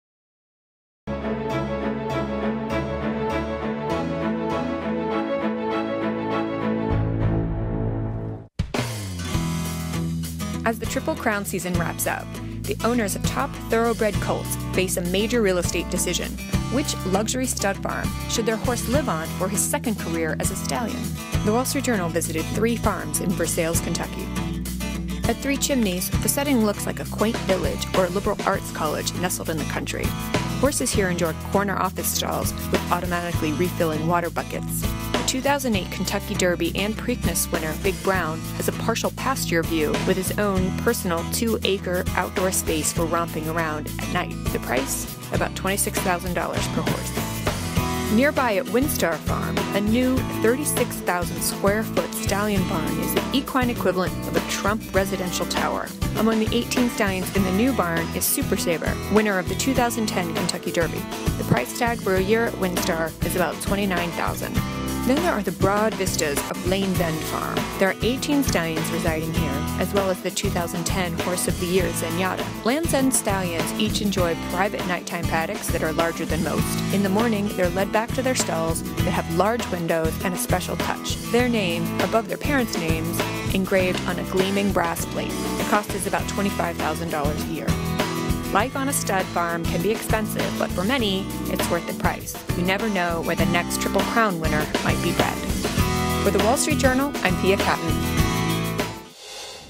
How a Champion Stallion Lives
A major real-estate decision looms for the top thoroughbred colts of the Triple Crown racing season: which luxury farms should they choose for their second career as a stallion? Pia Catton has a look at some  of the most eye-popping pastures.